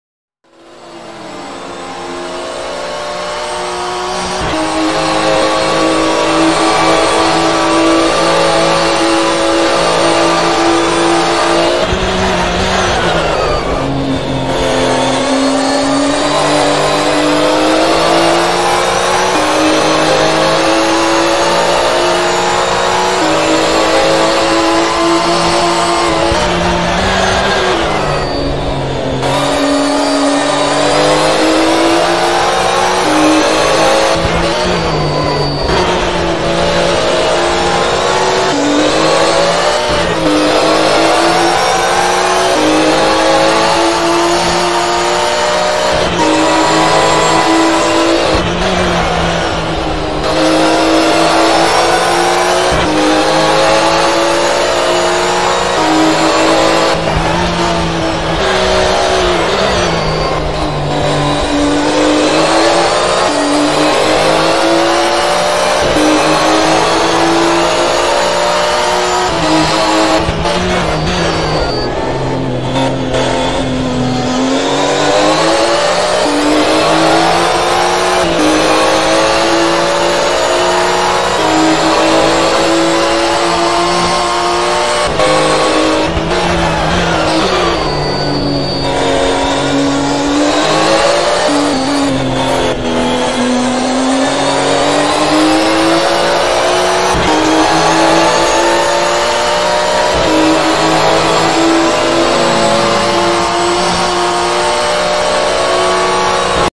IVRC 2021 GTC | Marc Stwerka Pole Lap at Imola
Qualifying Lap from Marc Stwerka at Autodromo Internazionale Enzo e Dino Ferrari with a lap time of 1:45.598 in a Porsche 911 GT3 R.